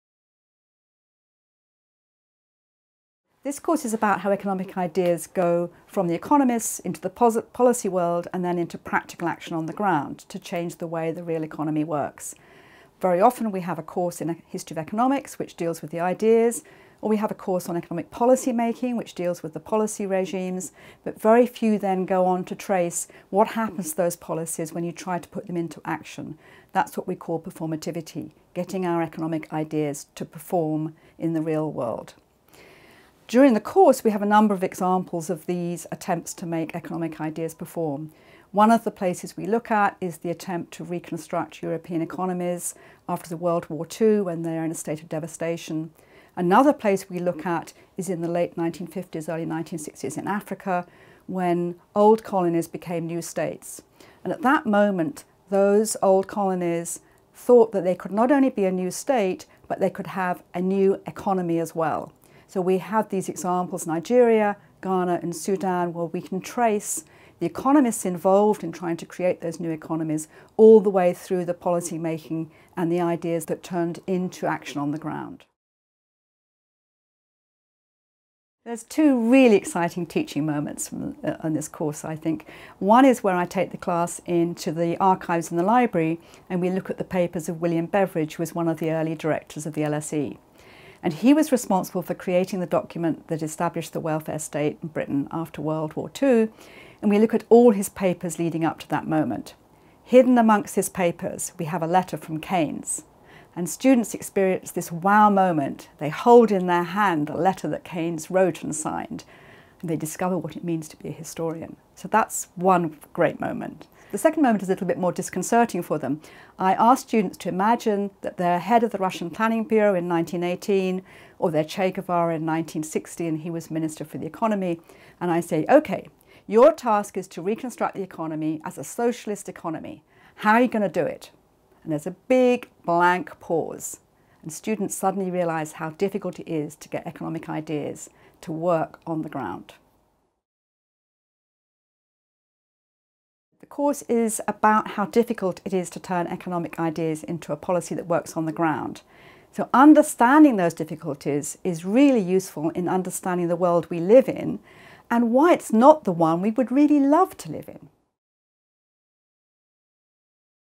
EH429 History of Economics: Ideas, Policy and Performativity
The course's aim is to understand how economics has been used to change the world.  The course will bring to together the long tradition of analysis of economics as a policy science with more recent ideas about the performativity of economics. It will draw on the literatures of economic history, history of economics and sociology of accounting and finance to explore the aims and methods used in economics to influence the economy.

The video is presented by Profesor Mary Morgan, Department of Economic History, LSE.